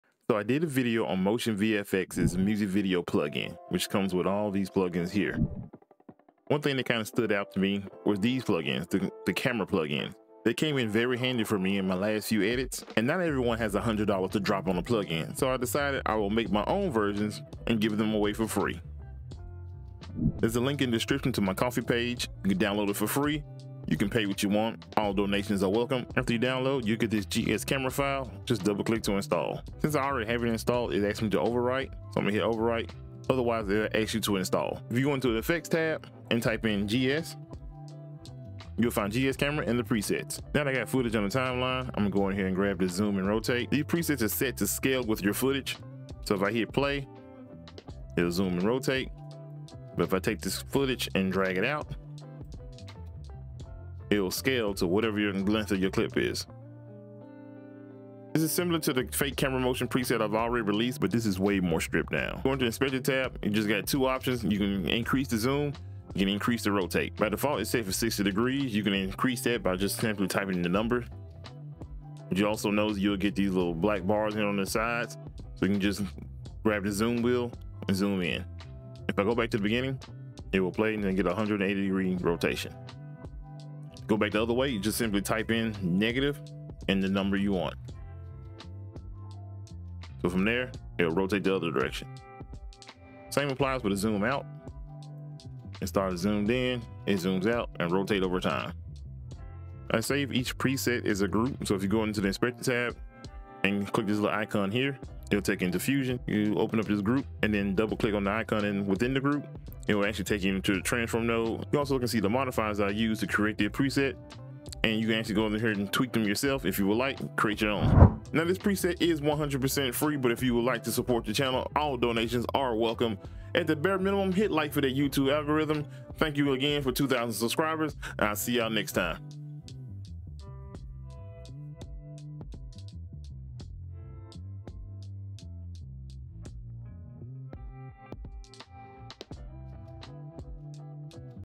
Free DaVinci Resolve 18 Preset
Free DaVinci Resolve 18 Preset that replicates the camera motion preset from motion VFX. link is below
link to MotionVFX Musicvideo plugin
MotionVFX 🎥🎥